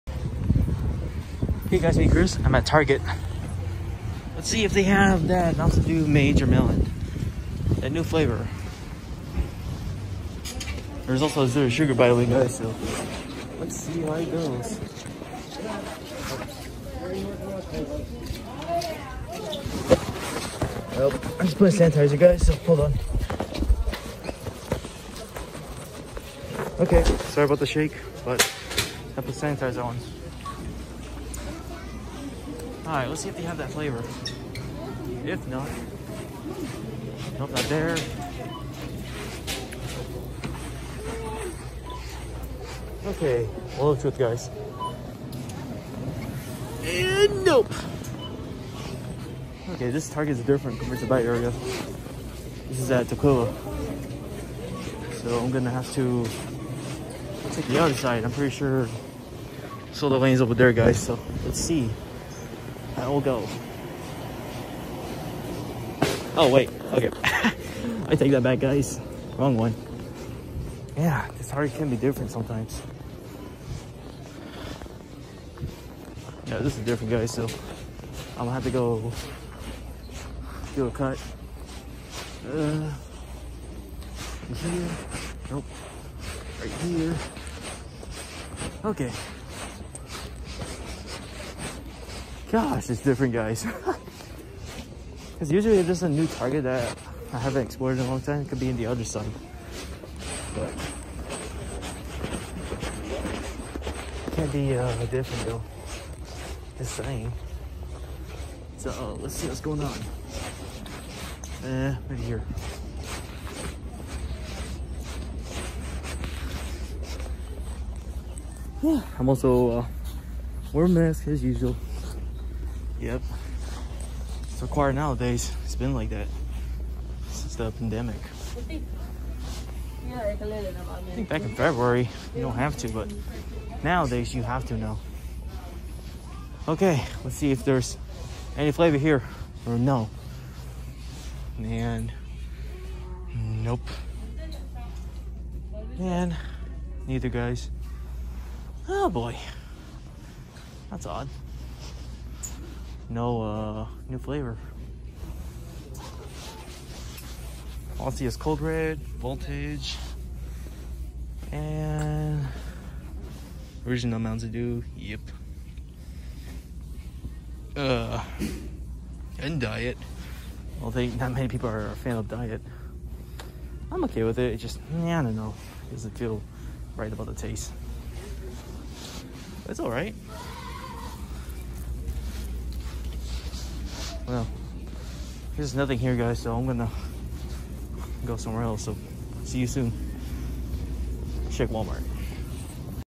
Searching For Mountain Dew Major Melon Episode 2 (Not Found)
Nothing at Target. I can still go to other places because there’s still hope of me finding that flavor.